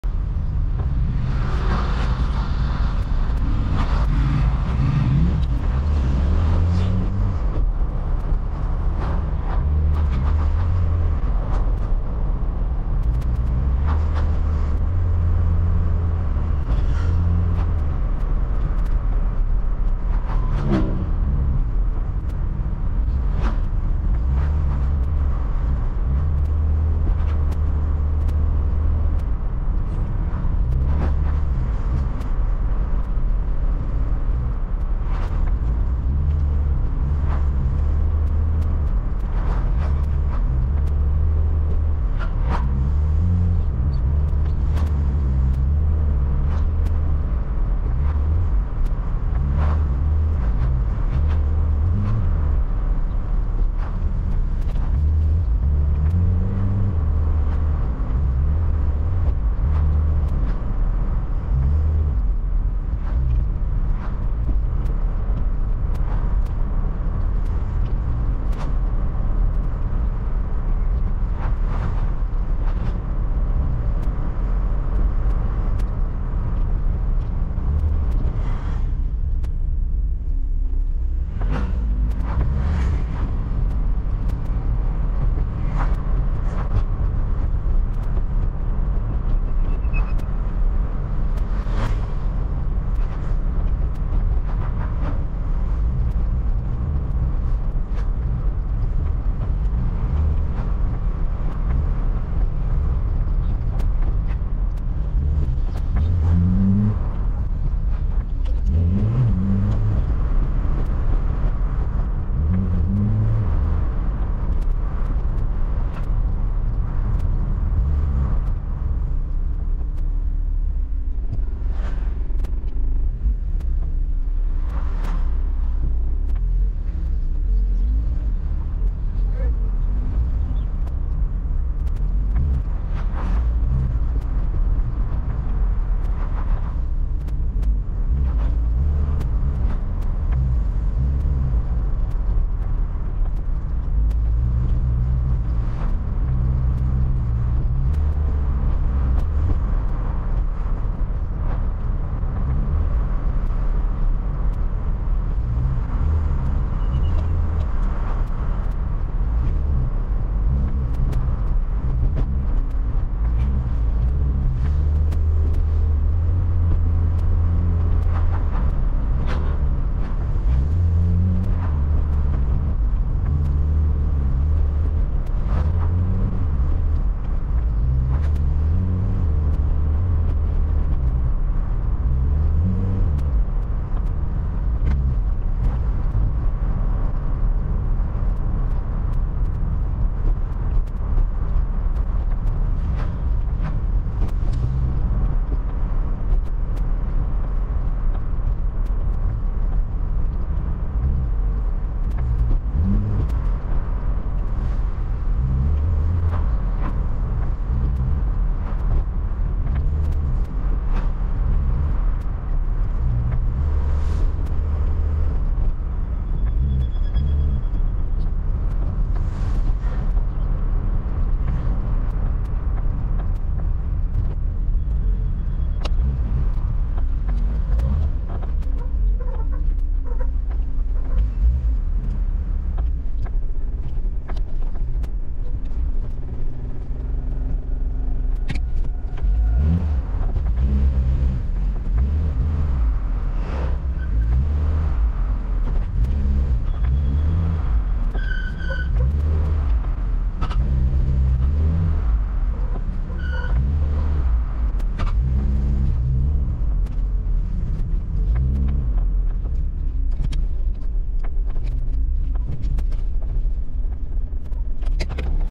省道 台9甲線 新店 - 烏來 6倍速度 縮時攝影 路程景
2022.4.4 省道 台9甲線 新店 - 烏來 6倍速度 縮時攝影 路程景